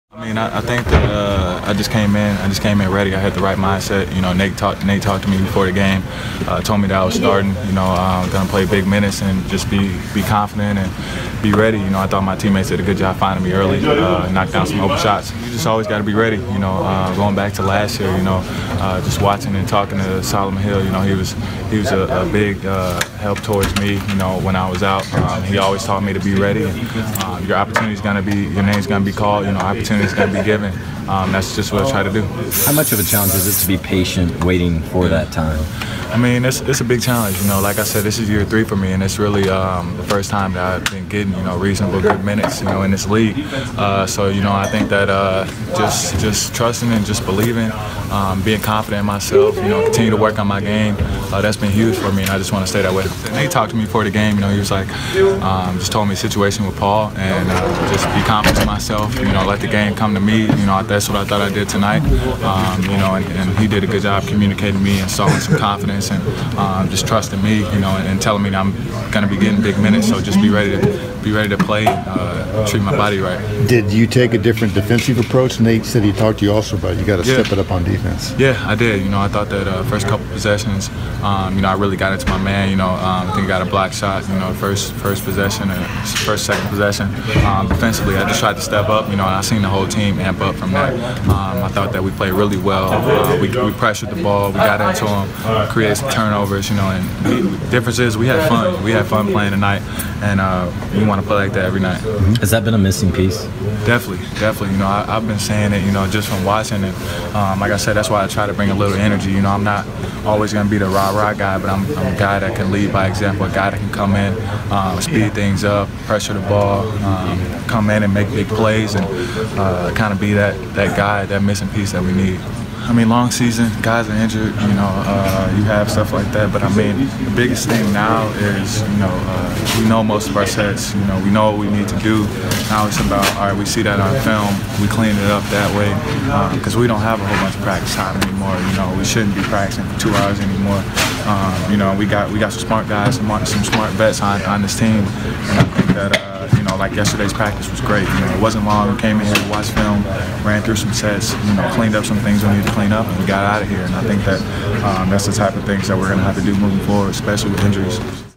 Glenn Robinson III after his career night
Glenn Robinson started in place of the injured Paul George and turned in the best game of his three-year career: 20 points and six rebounds in almost 40 miuntes. Afterwards, he discussed it, how he remained patient, and his approach going forward.